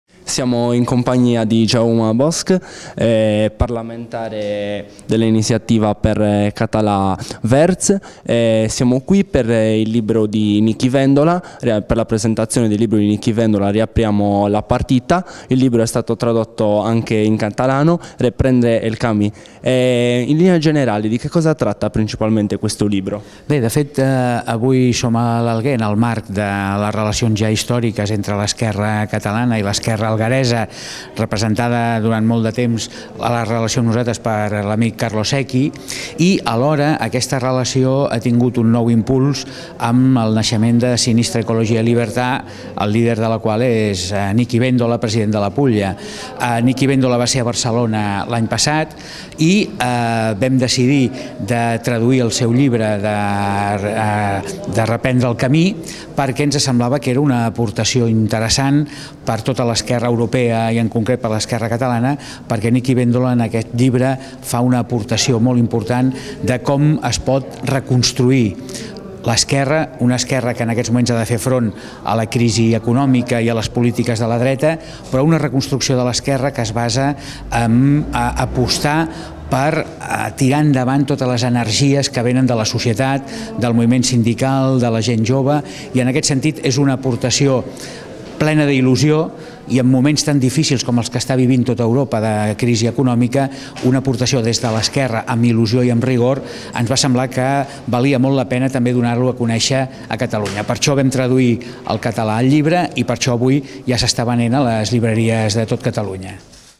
Jaume Bosch : Reprendre el camì
Intervista a Jaume Bosch, parlamentare de Inciativa per la Catalunya -Verdas, sul libro di Nichi Vendola " Riaprire la partita" tradotto in catalano.